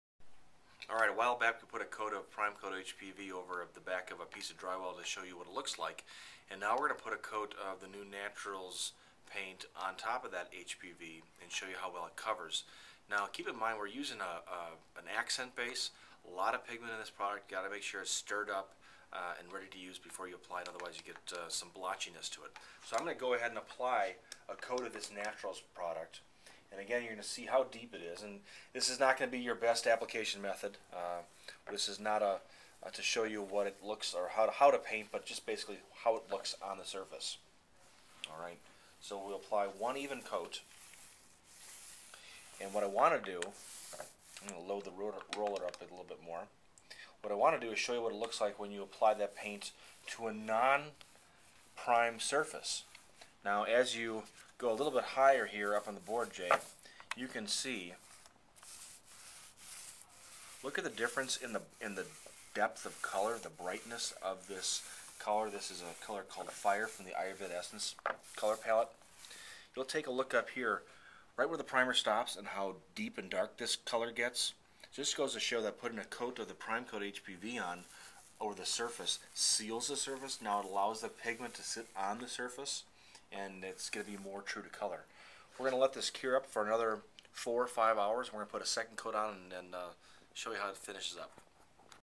NaturalsOverPrimecoat.MP4
Andy Pace of Green Design Center in Waukesha WI discusses AFM Safecoat
Naturals Plant Based Oil Paint over Safecoat New Wallboard Primer.